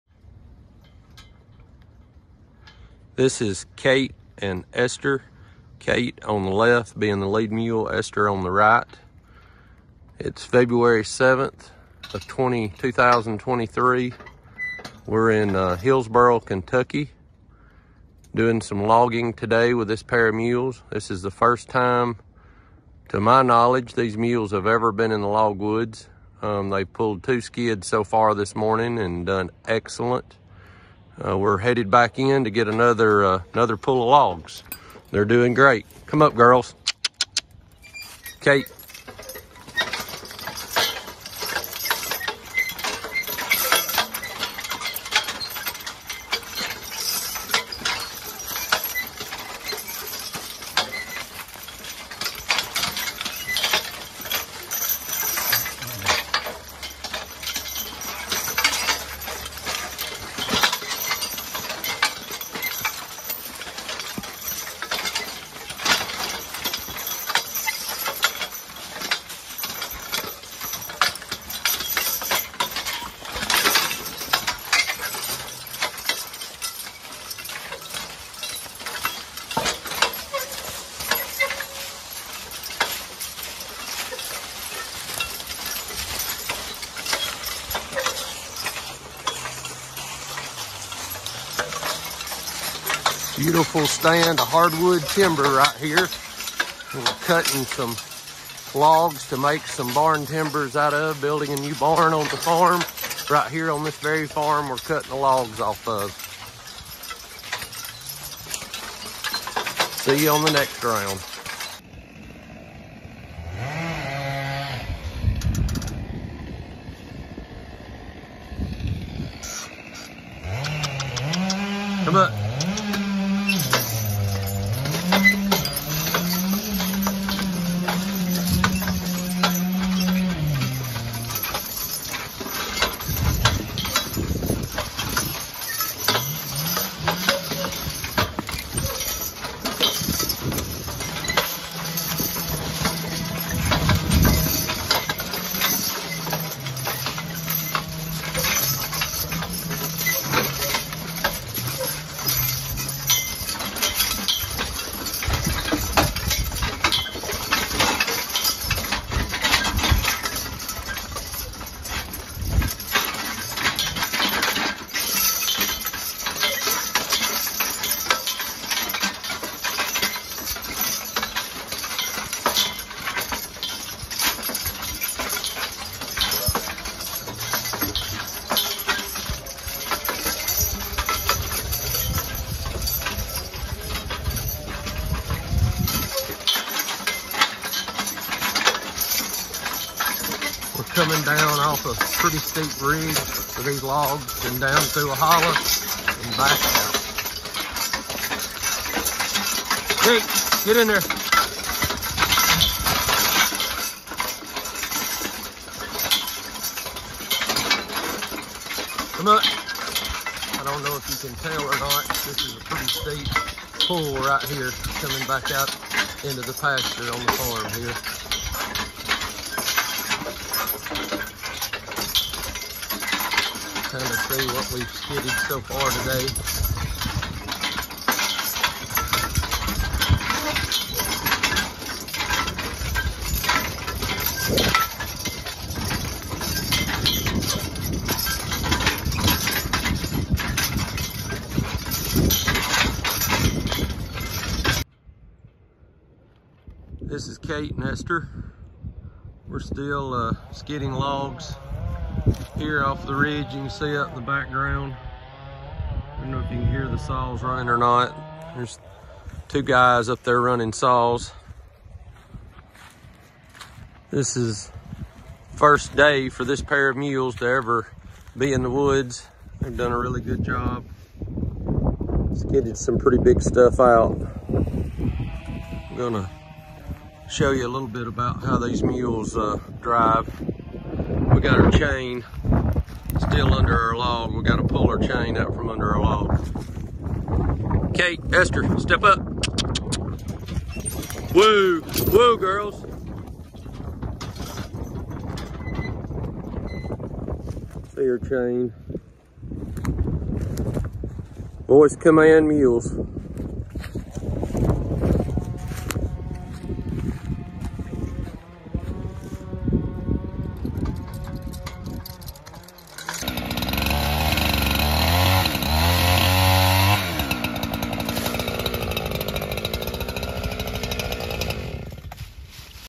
Mule Jam 2023 Team 1
Voice command, big blonde girls, gentle, will be available at Mule Jam 2023 Team 1.  August 4 & 5 2023. Russellville KY Ag Center.